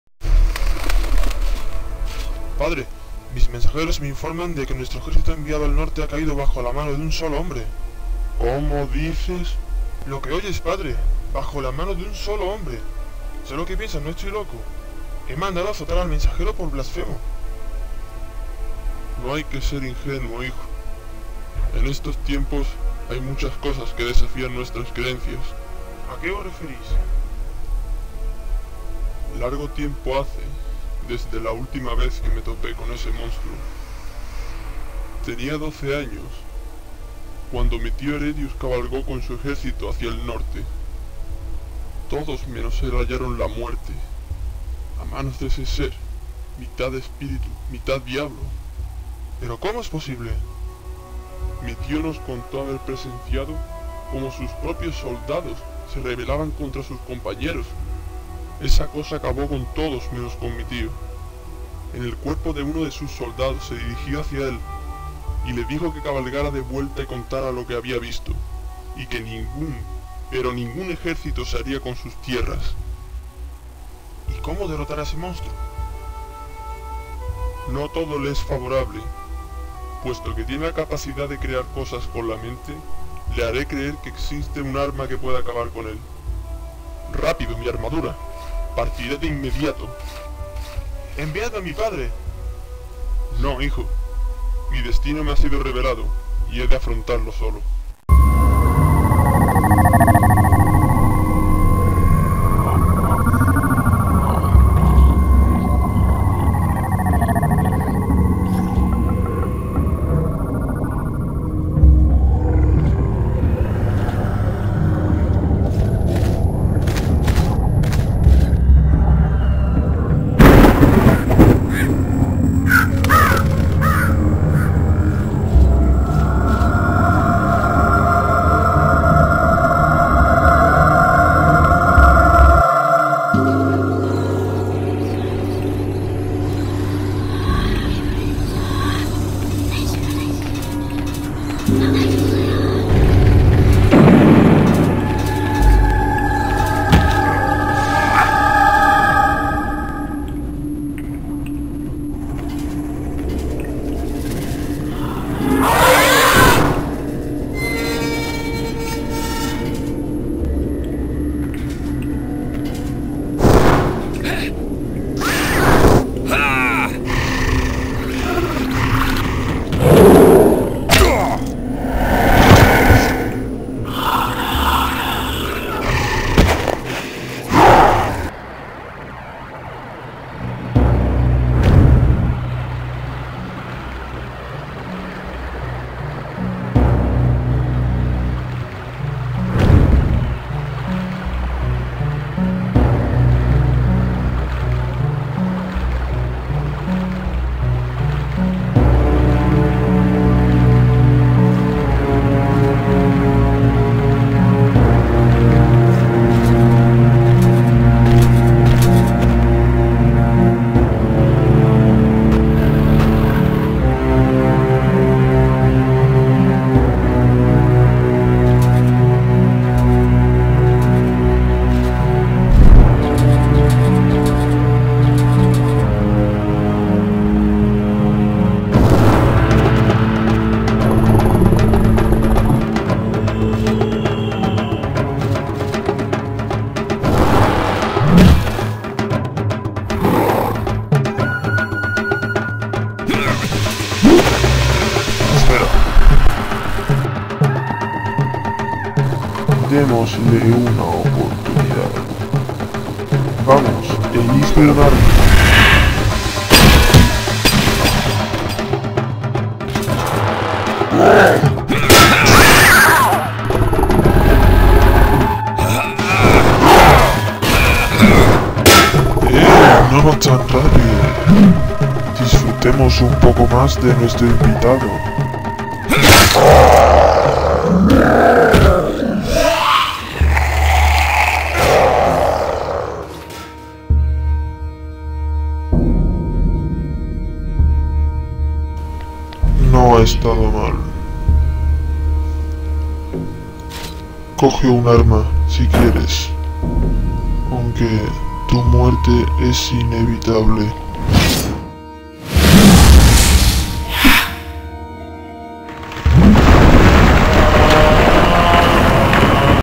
animacion caballero tiflos
An animation made with max, combustion and photoshop, about a knight looking for a kind of human-alien in a weird castel, inspired in WoW and blade (the videogame)